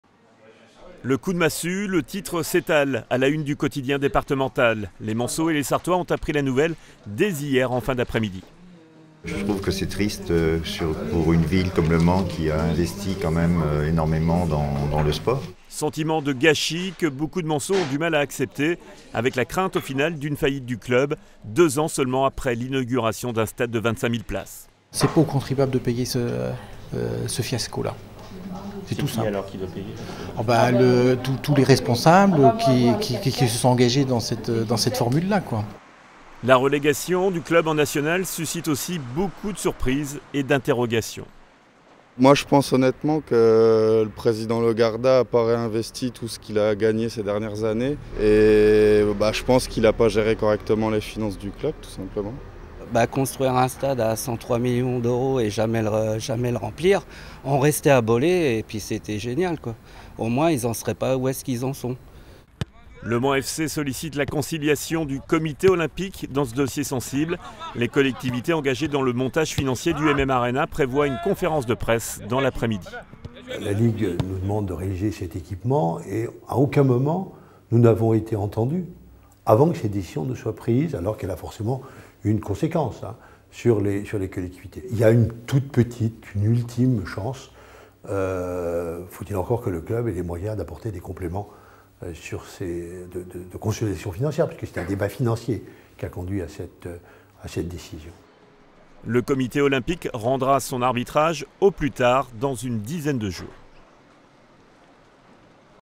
Le Mans FC rétrogradé en National ! Les réactions
Un choc !  Le club du Mans FC est rétrogradé en National par la commission d'appel de la Direction nationale de contrôle de gestion. Une première pour un club ayant validé sportivement son maintien en Ligue 2. Une décision lourde de conséquences pour une ville dotée d'un stade flambant neuf de 25000 places.
Reportage France 3 Maine